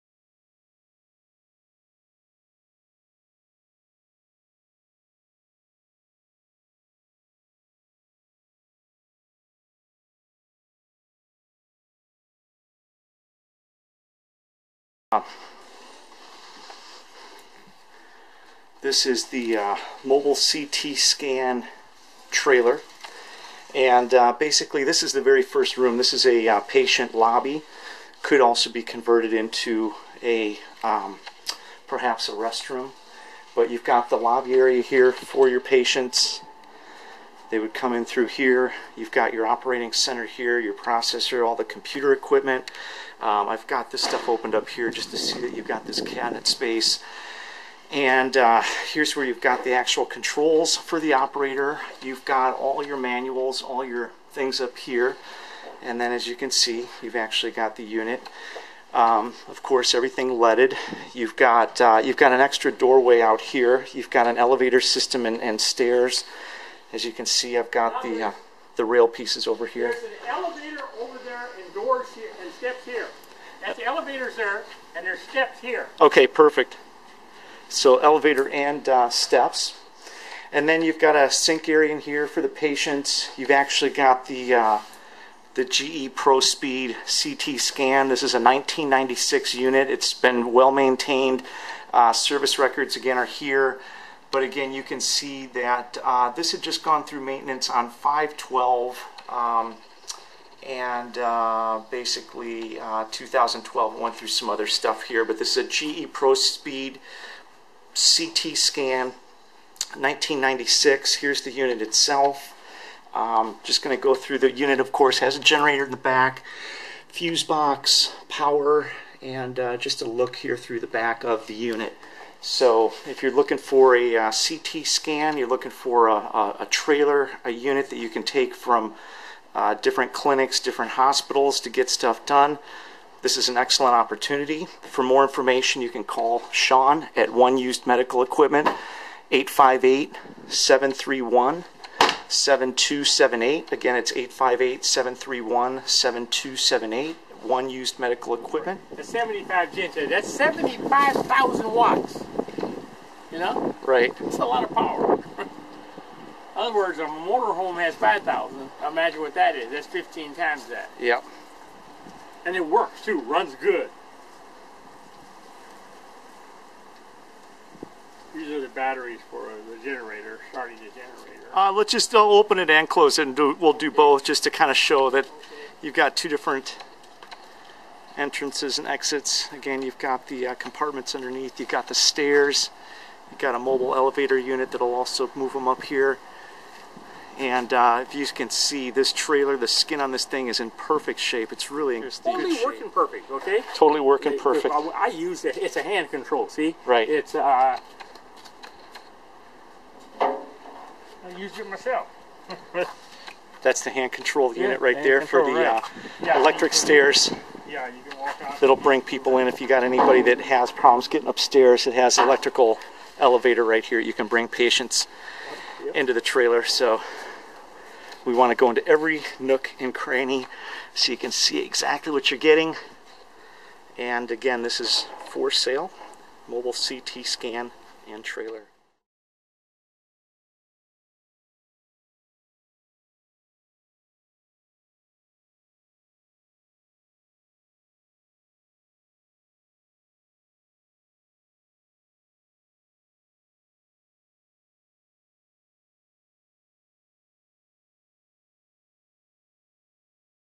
1996 Prospeed CT Scan and Mobile Medical Trailer for Sale San Diego
Order - price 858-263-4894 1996 Prospeed CT Scan and Mobile Medical Trailer MOBILE CT SCAN FOR SALE - 1996 GE Prospeed CT scan complete with new tube and 45 foot custom medical trailer with 75,000 amp generator $28,500.00 USA - The trailer is in excellent condition and includes stairs and a mobile hydrolic lift for patients that cannot get up stairs. This medical trailer comes with an official bill of sale from the hospital for registration of the trailer unit with DMV if necessary. If you are looking for a used CT scan this is a great opportunity to have a mobile unit that does not need installation and can go to other locations and not take up space inside a hospital or clinic. Used 1996 Prospeed CT scan has brand new tube ready for 2-3 million single slice frames.